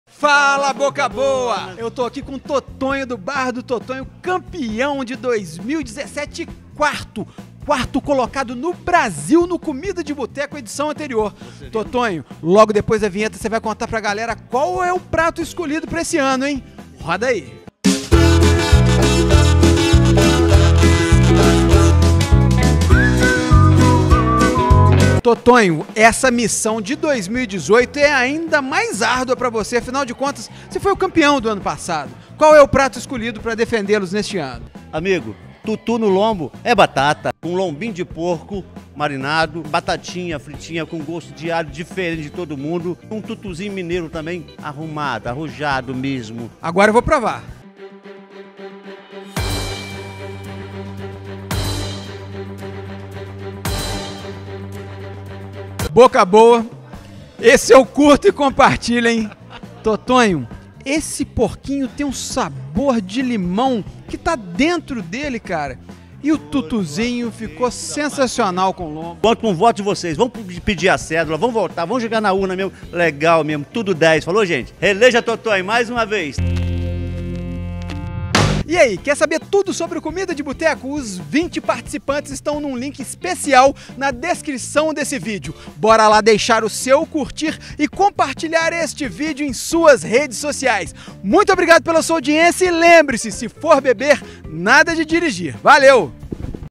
Bar do Totonho apresenta TUTU NO LOMBO É BATATA | Zine no Comida di Buteco 2018
TUTU NO LOMBO É BATATA: Tiras de lombo empanado, acompanhados de batata frita e tutu à mineira. Acompanha especial molho da casa.

🏡 Bar do Totonho: Rua Carlos Tavares de Pinto, nº 50 - Retiro (Juiz de Fora/MG)
⌚ Funcionamento: Terça à Sexta das 17h às 00h | Sábado das 12h às 00h | Domingo das 11h às 16h